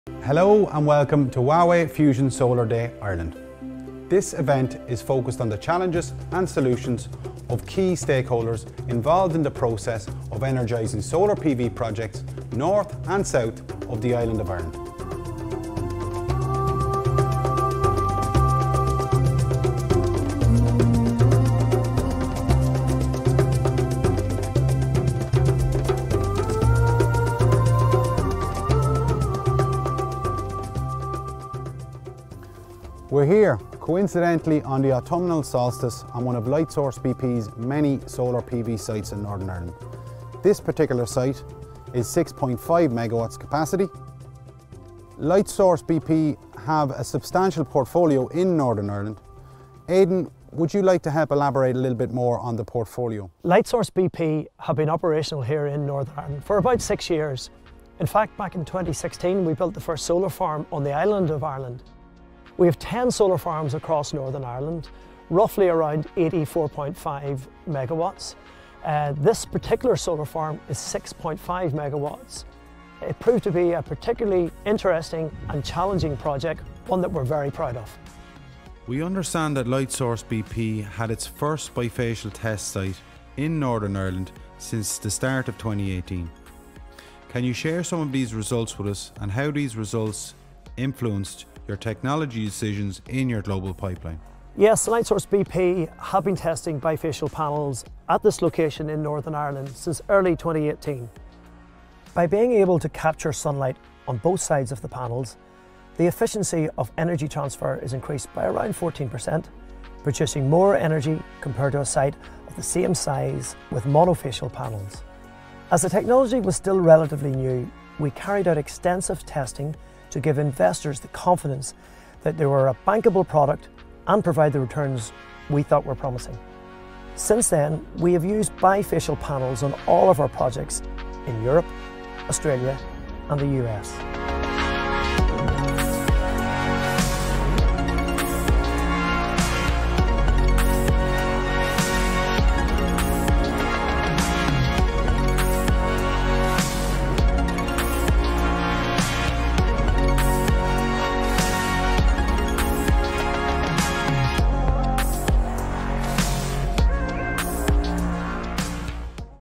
#2 Huawei Smart PV FusionSolar Day Ireland 2020: Aidan Collins - Lighsource bp
112 MW Utility-scale over 6 sites in Northern Ireland